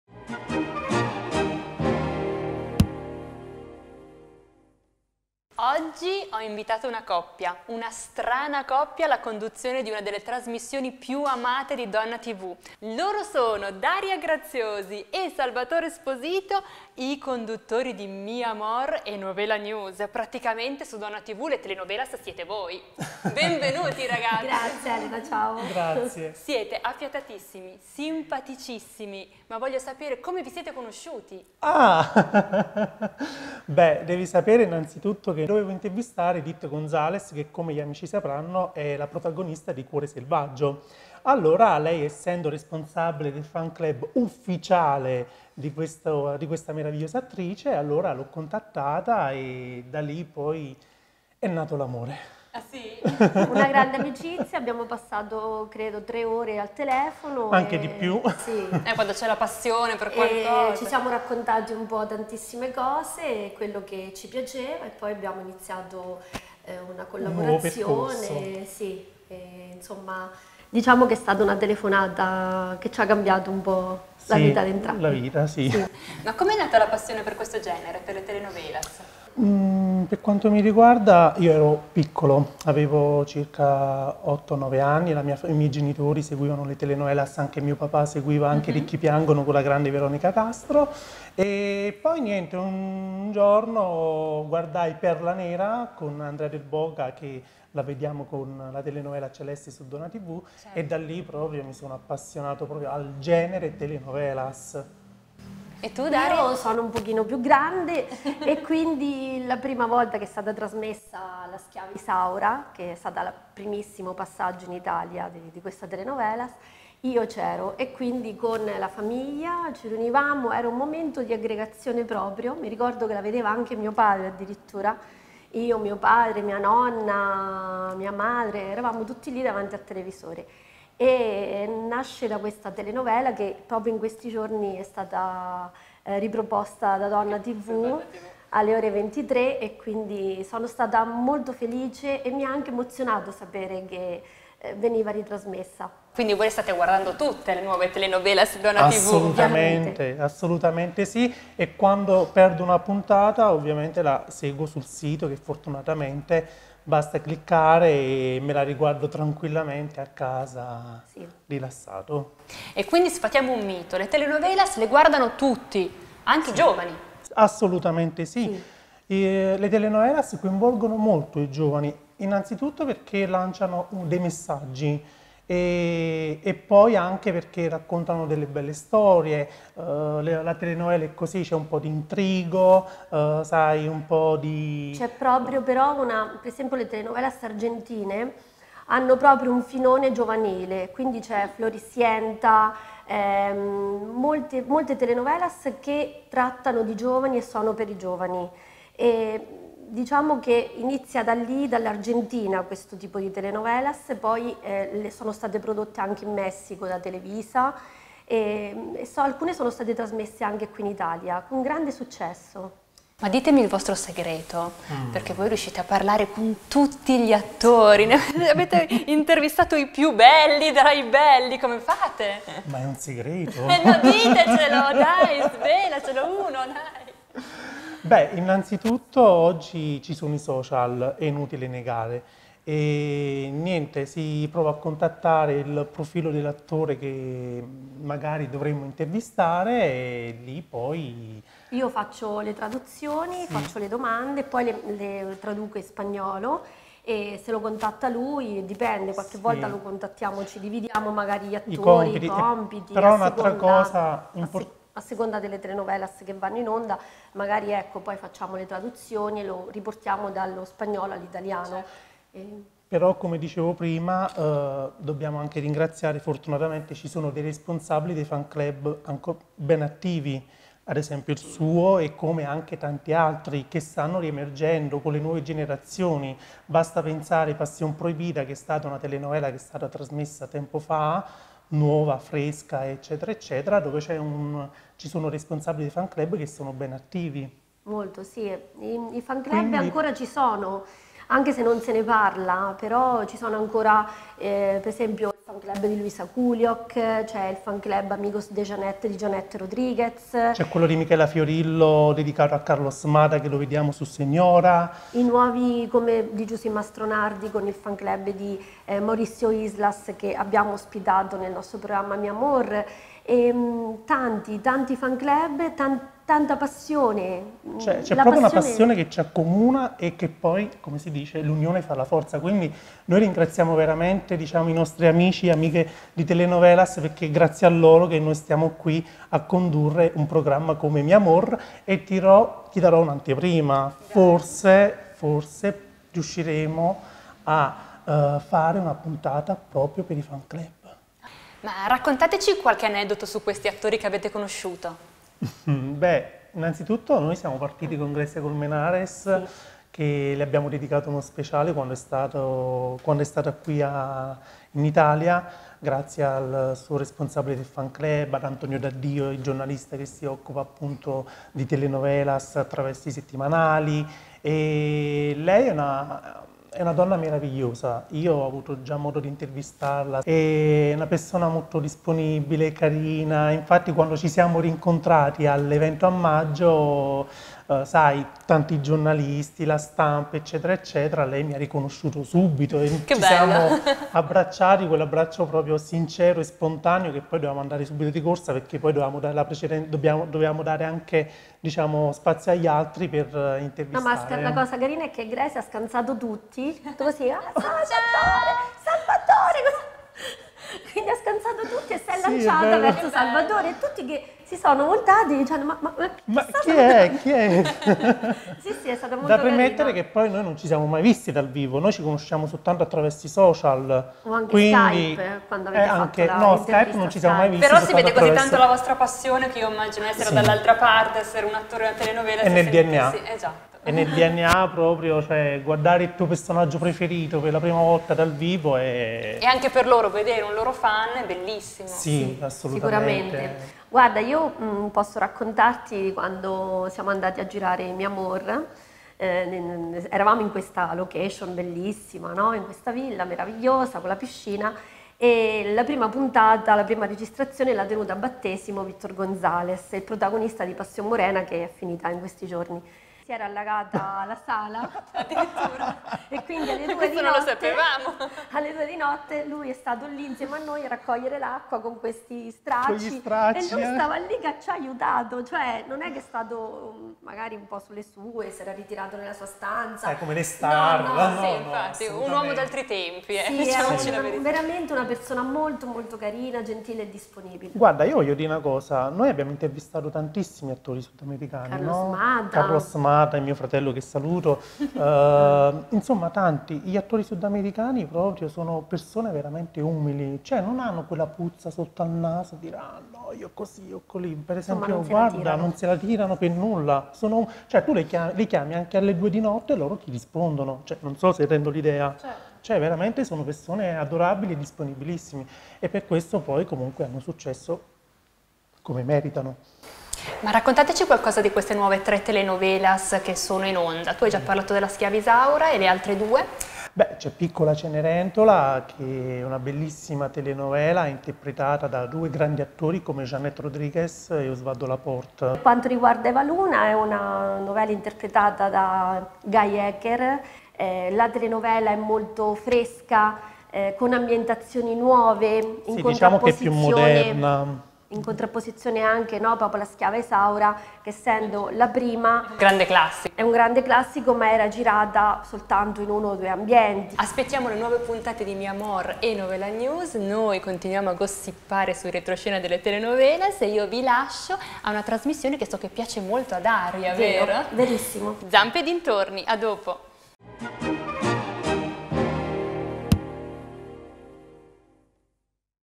DONNA PRATICA INTERVISTA A DARIA E SALVATORE
Elena Maria Manzini intervista Daria & Salvatore.
Scoprite CHI SIAMO!